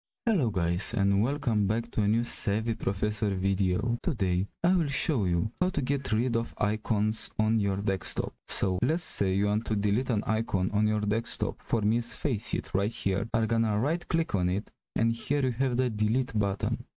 How to Get Rid of Icon on Desktop (Delete Icons on Desktop)
How to Get Rid of Icon on Desktop (Delete Icons on Desktop). In this tutorial, you will learn how to get rid of an icon on desktop.